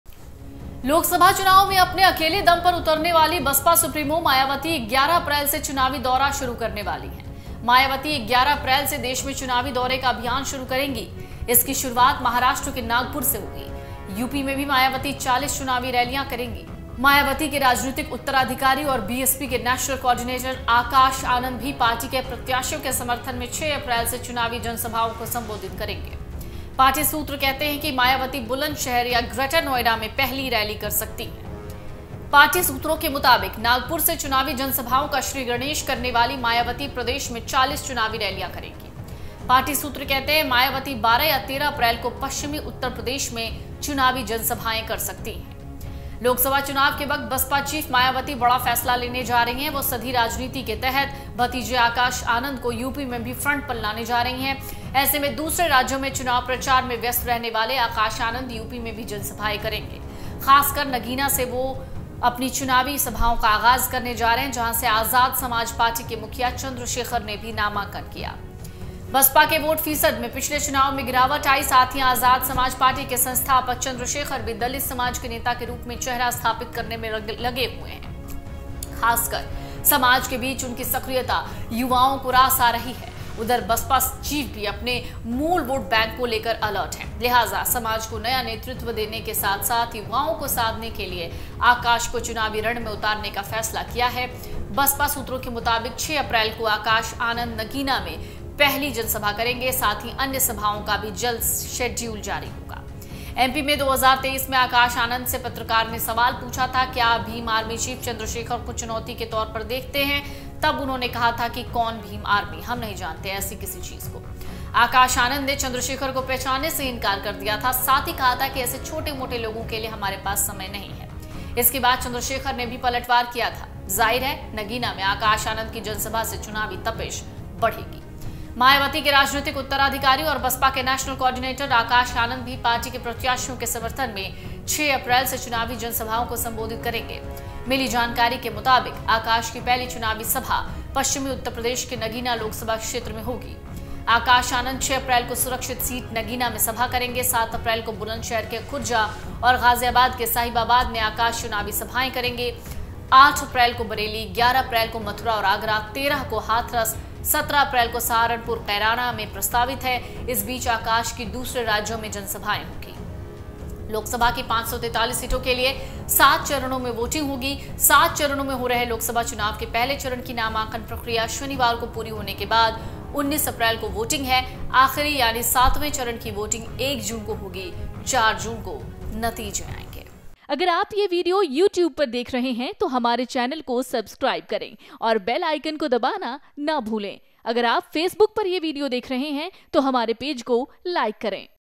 UP Politics: UP में Mayawati का बड़ा प्लान! Akash Anand को उतारकर साधेंगी मैदान? Loksabha Election
मायावती और आकाश करेंगे प्रचार, नगीना से होगा चुनावी रैली का आगाज, आकाश आनंद को 'रण' में उतारने की तैयारी, मायावती भी करेंगी धुआंधार प्रचार